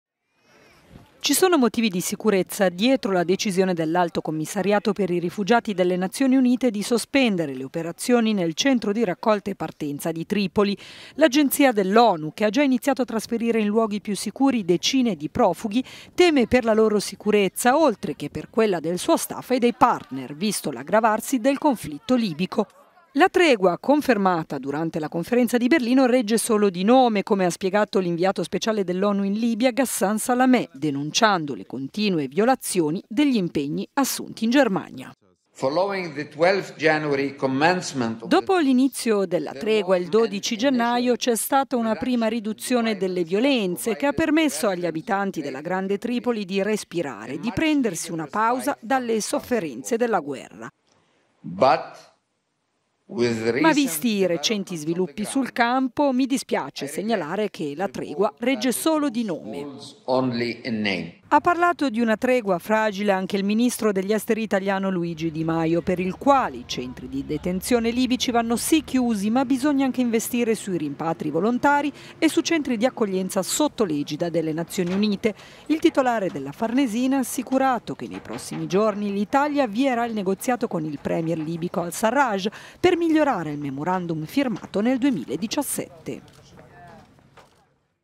La tregua regge solo di nome. UNHCR sospende le attività nel centro rifugiati di Tripoli
L'Agenzia Onu, con l'aggravarsi del conflitto, teme per la sicurezza dei profughi, ma anche dello staff e dei partner…